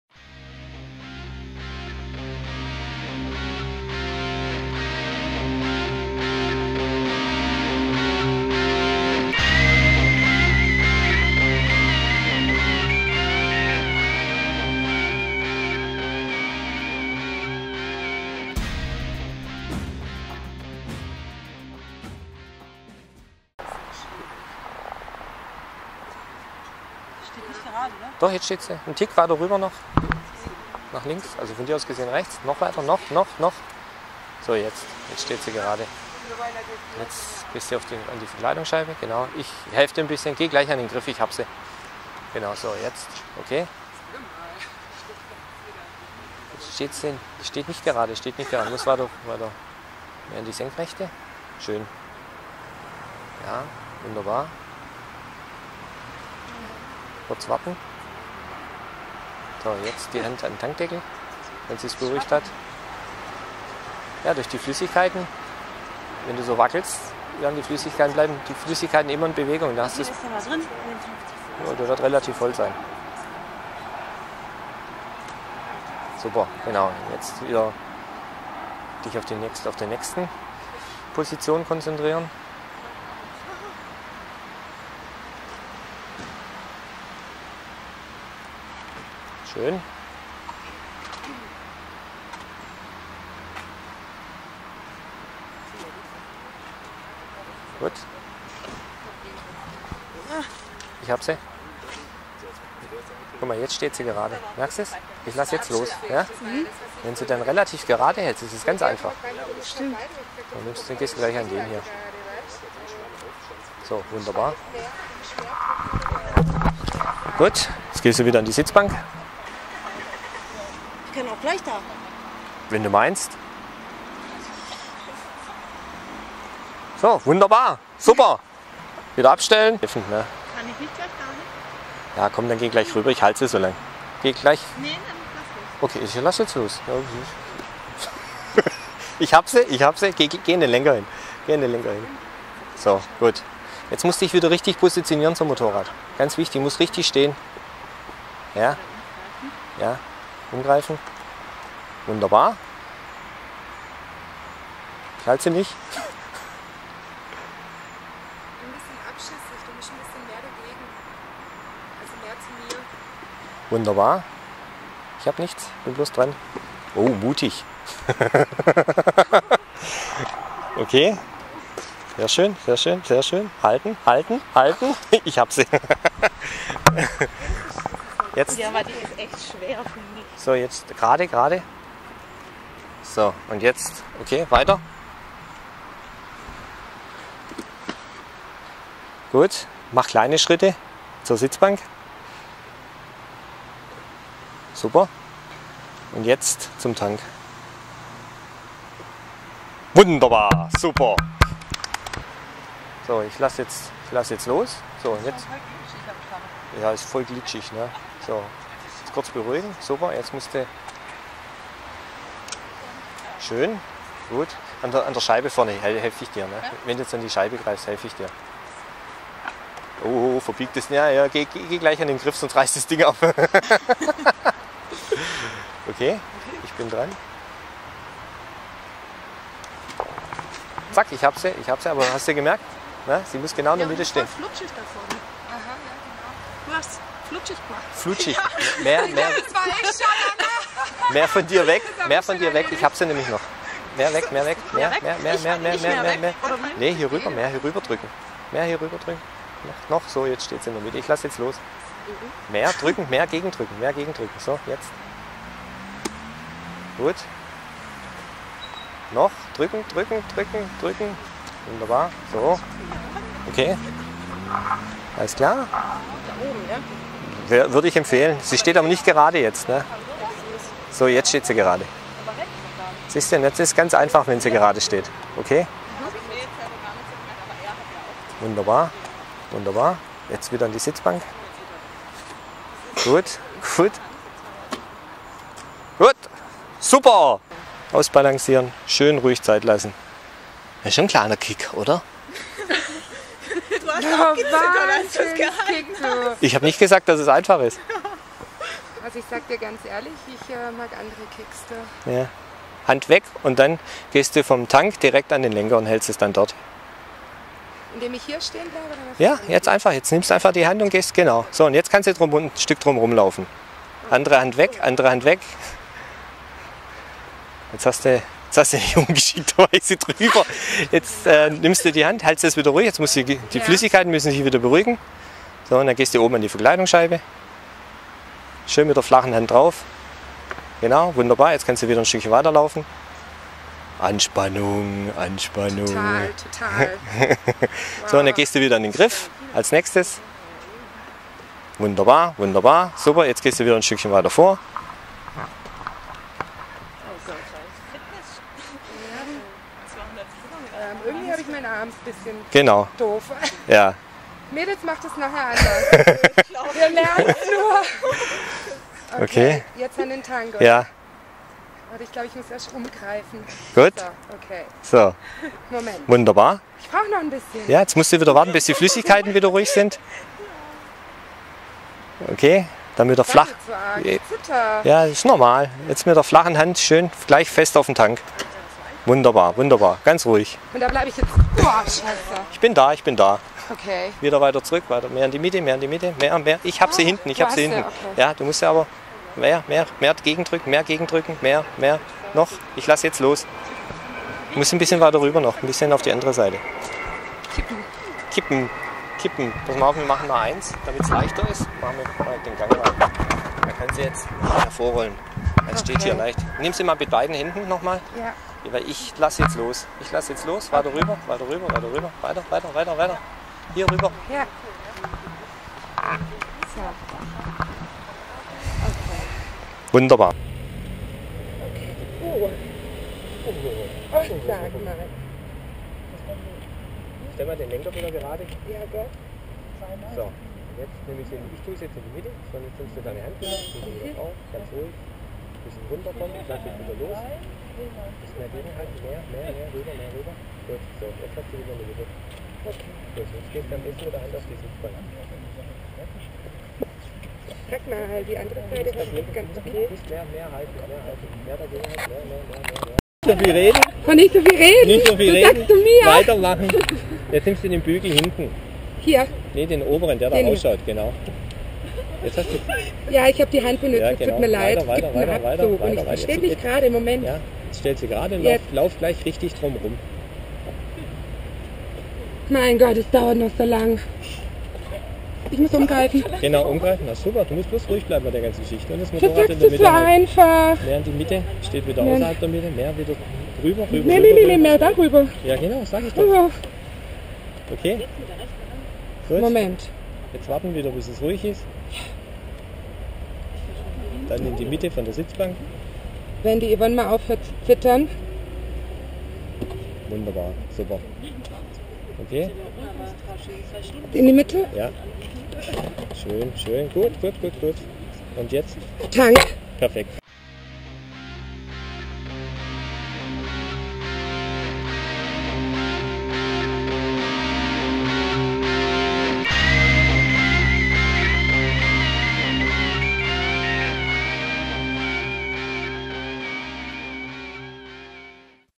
MOTORRAD Ladies-Cup - Übung 1: Suzuki balancieren
Vier Führerscheinanwärterinnen haben die Aufgabe, eine Suzuki Gladius zu umlaufen und sie dabei mit einer Hand zu balancieren. Stimmen Sie ab, wer diese Aufgabe am besten gelöst hat.